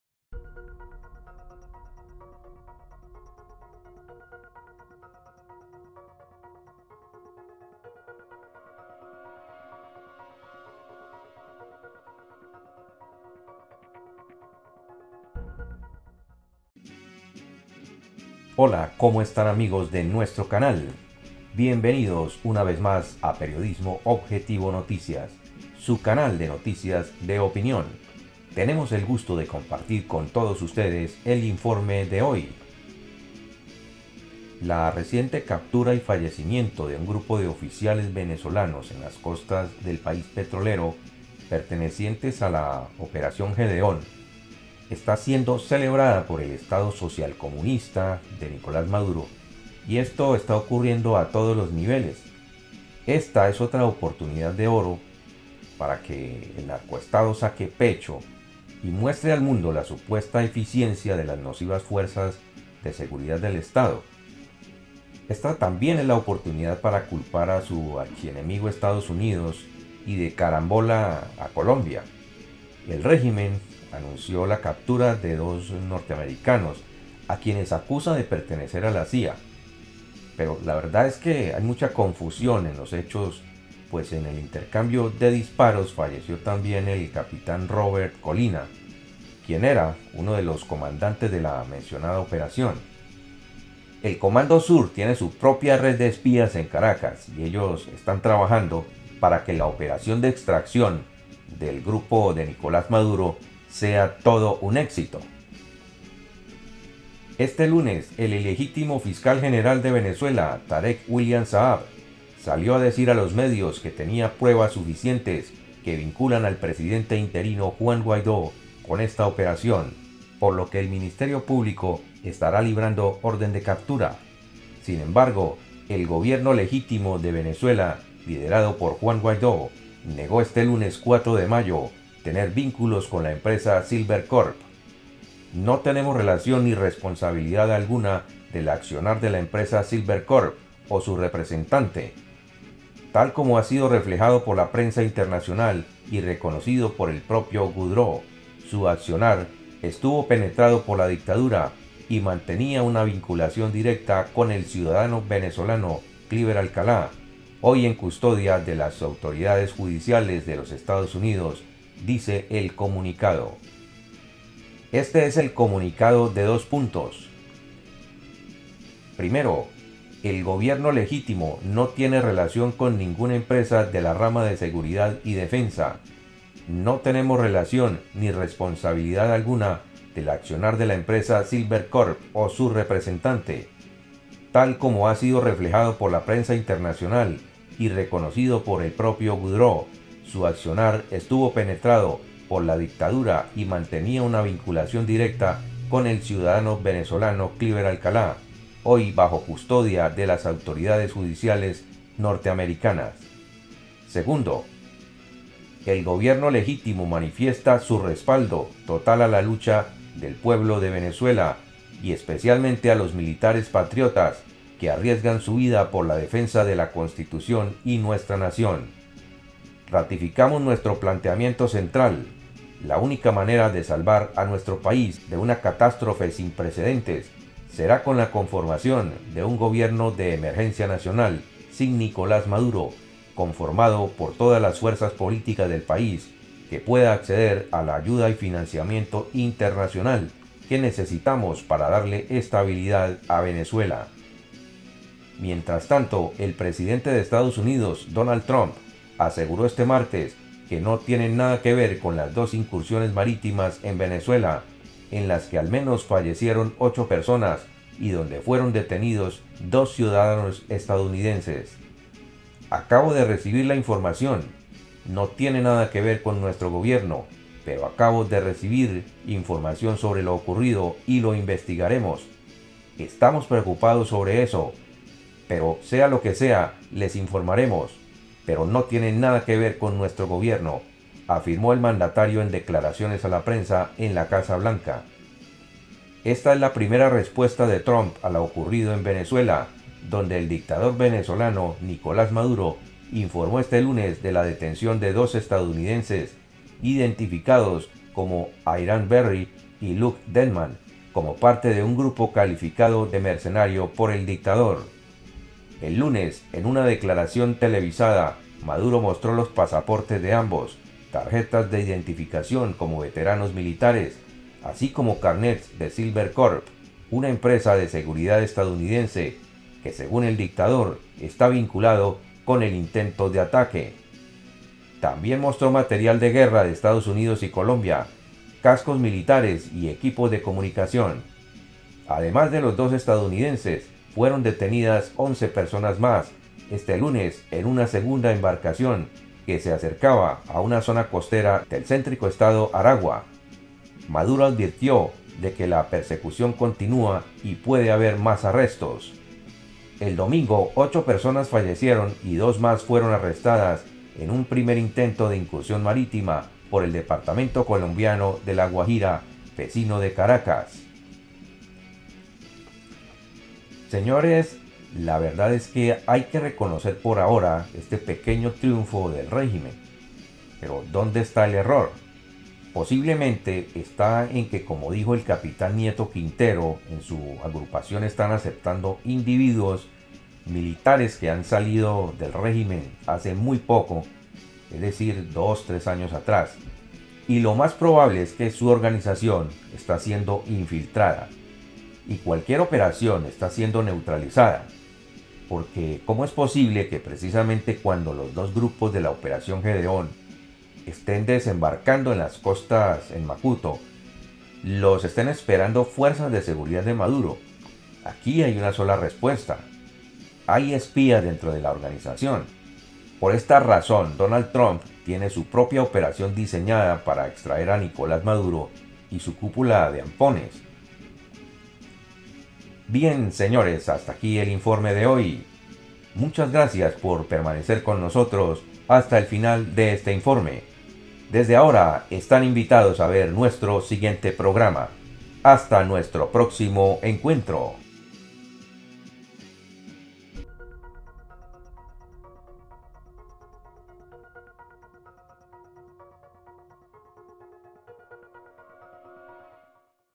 TRUMP TIENE EN MARCHA EL OPERATIVO DE EXTRACCION CONTRA MADURO !
El presidente de Estados Unidos Donald Trump pune en marcha el Operativo para extraer a Nicolas Maduro. Sera un operacion limpia.